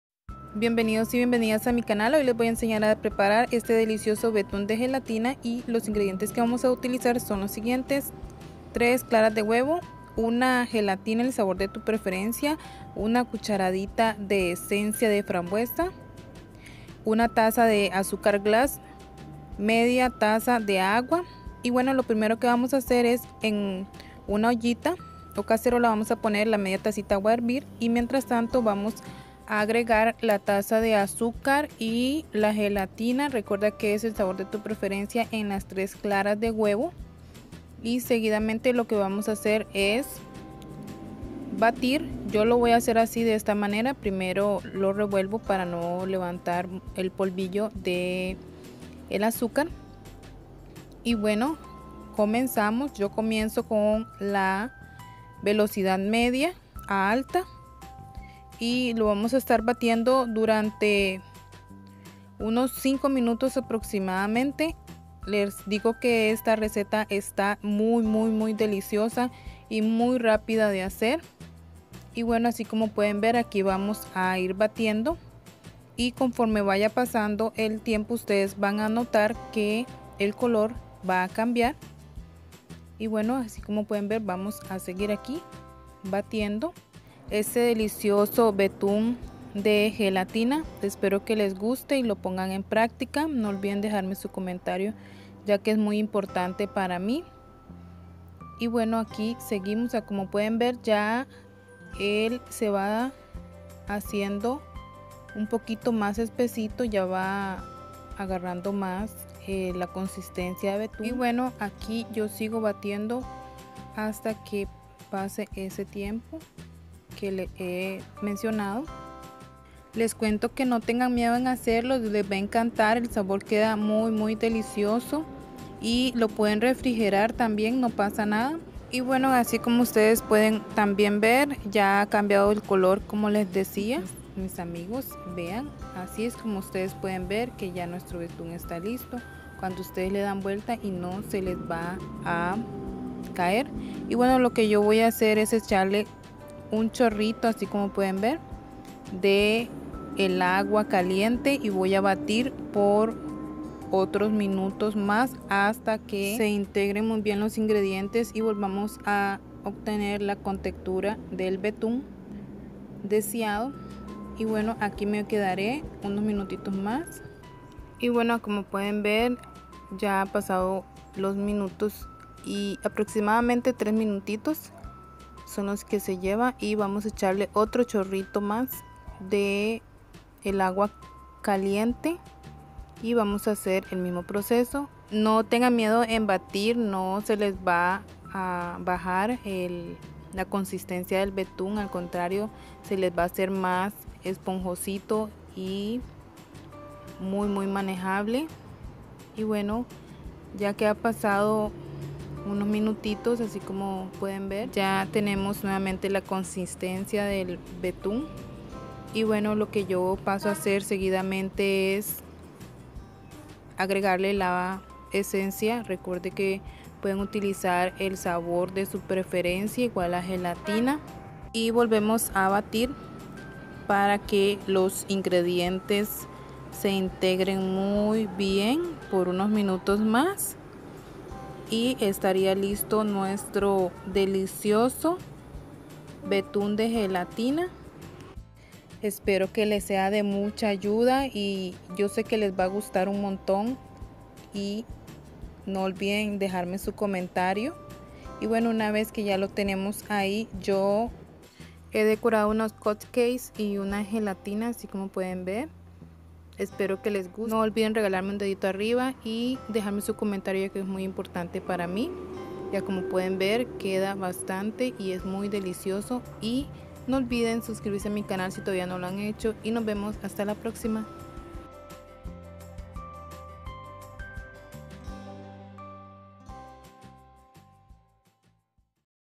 Betún de gelatina / Merengue (Betun) de Gelatina / Cocina con Jenny
En este video te enseño Como hacer Betún de gelatina / Merengue (Betun) de Gelatina, frosting de gelatina para pateles o cupcakes